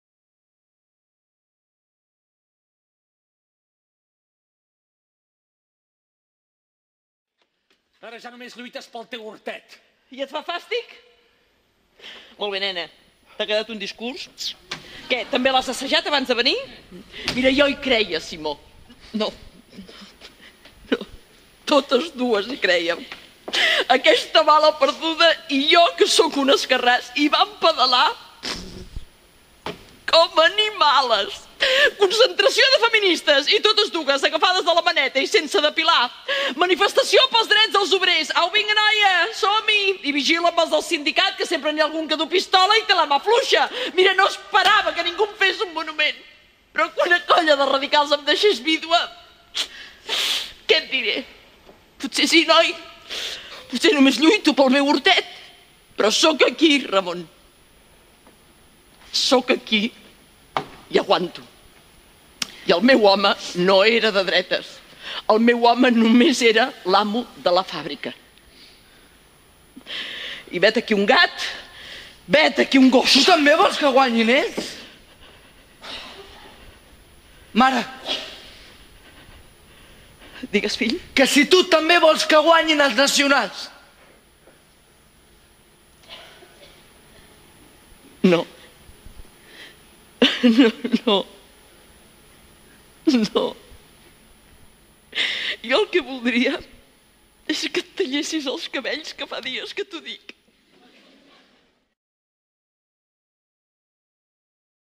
Barcelona (fragment)
Fragment de l'obra de Pere Riera, Barcelona que es va estrenar el maig del 2013 a la Sala Gran del TNC. Aquesta gravació va ser emesa per tv3.

Núria: Míriam Iscla